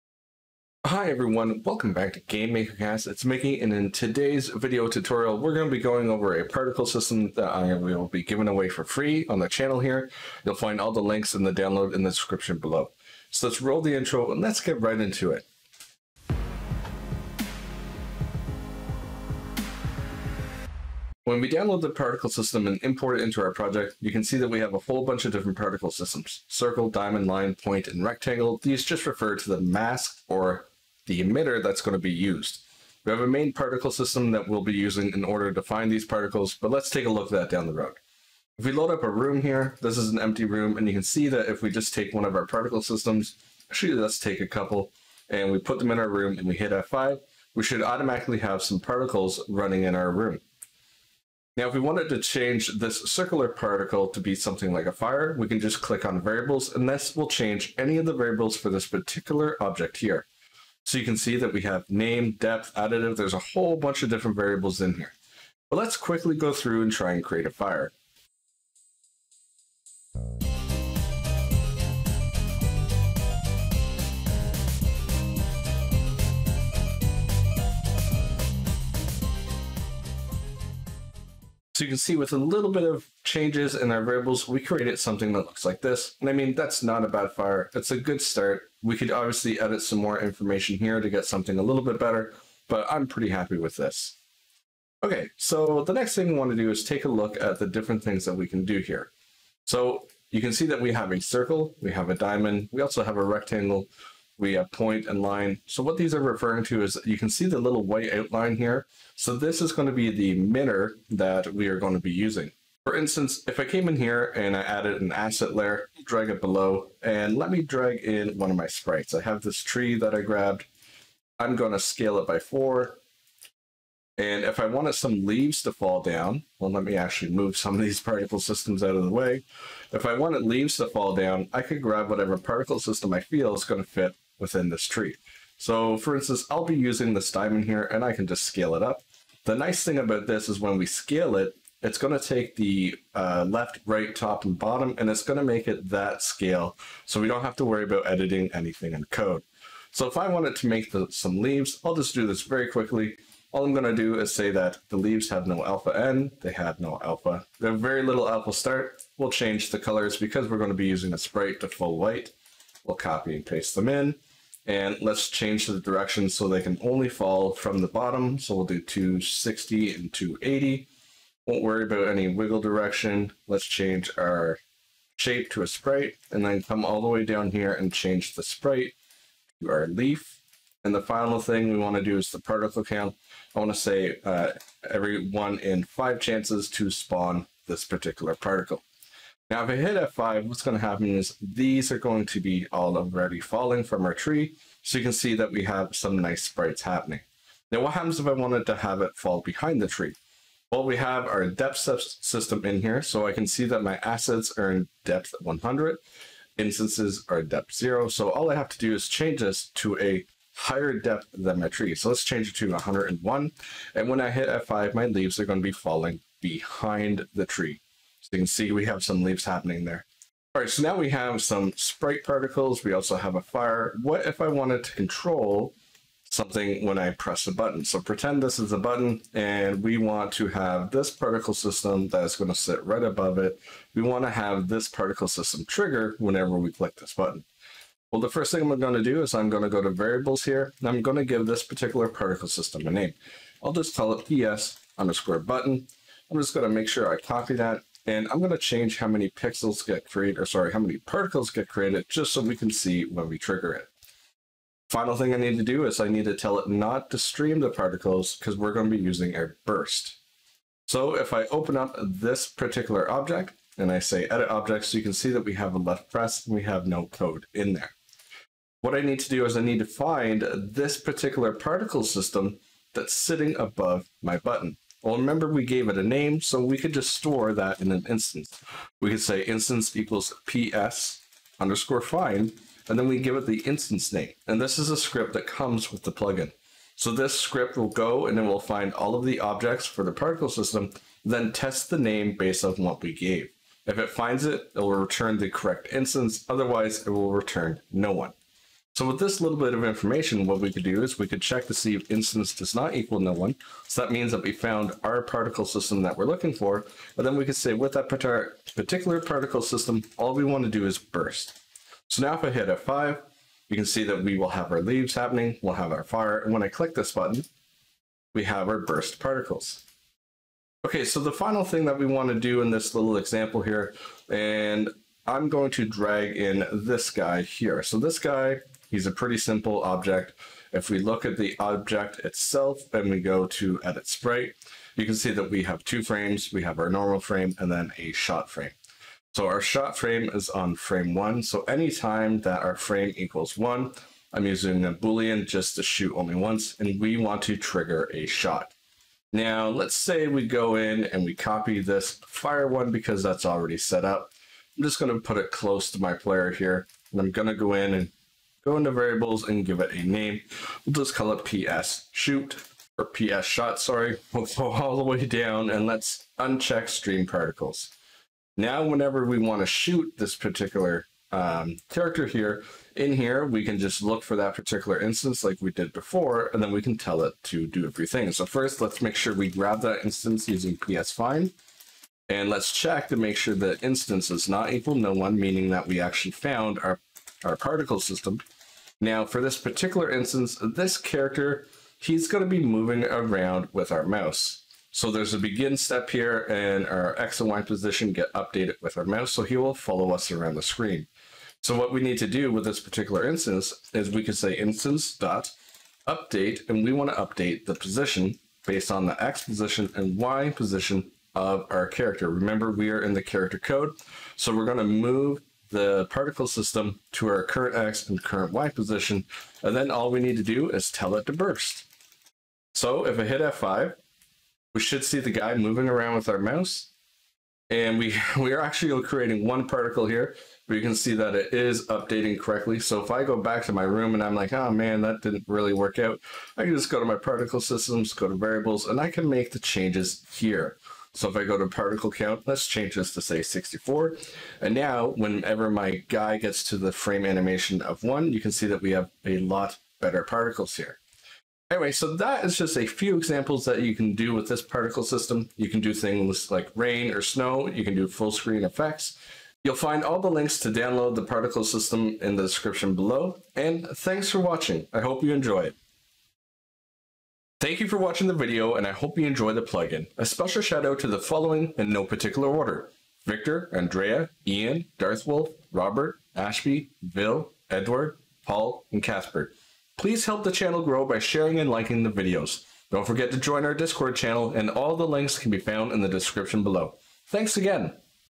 GameMaker Studio 2 - Free Plugin - Particle System
In this video we will go over a free plugin that I am releasing. It is a simple particle system that allows you to skip the coding of the particle systems in GameMaker Studio and create them in the room editor.

This video will walk you through a few examples that you can create with this system but the possibilities are endless.

[ Video Contents ]

# 00:00 - Introduction
# 00:25 - Particle System Overview
# 00:54 - Creating a Fire Particle System
# 00:40 - Emitters
# 01:55 - Sprite Particles
# 03:18 - Depth System
# 03:45 - Bursting Particles
# 05:25 - Changing Particle System Coordinates
# 07:45 - Thank you!

[ Sources ]
--- Art ---